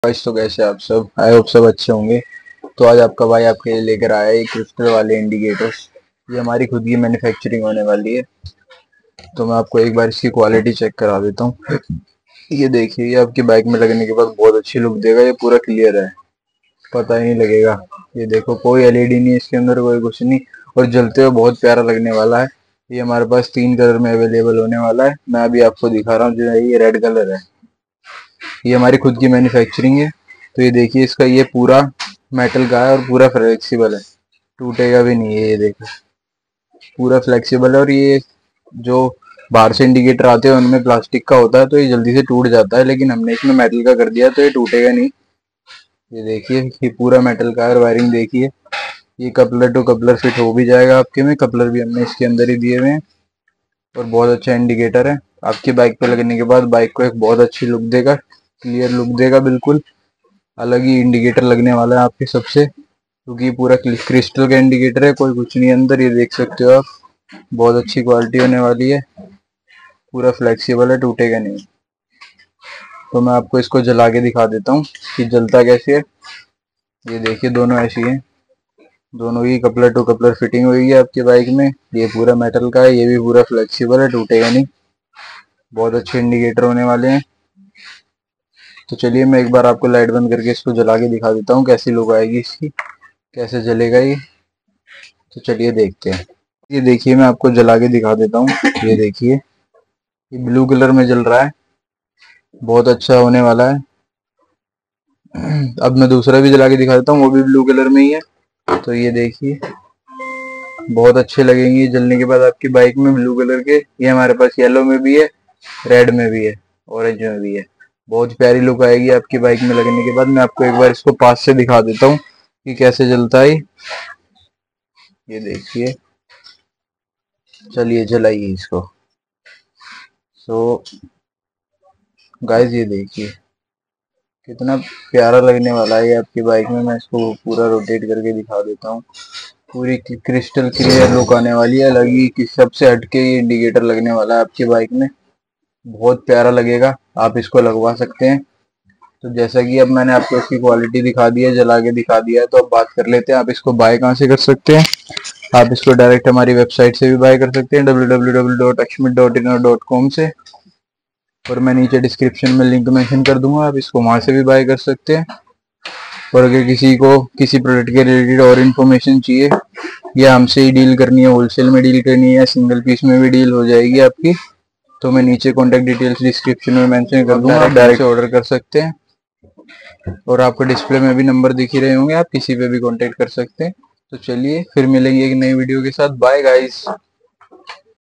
Bike Fancy Crystal Indicator | Modification Indicators Review | Aksmit Bike Accessories Wholesale
Compatibility: 12V LED Motorcycle Turn Signals Indicator.
High quality: Motorcycle turn signal is made of ABS plastic, which has high transparency and good wear resistance that guaranteed by quality Universal Fit: For all kinds motorcycle, dirt bike body accessories, turning lights modification.
Excellent Performance: It features high brightness, low power consumption, high efficiency and stable performance.

Made for your bike and give an attractive look to your bike. 😱

• WhatsApp family - ‎Follow the Car and Bike Accessories Aksmit channel on WhatsApp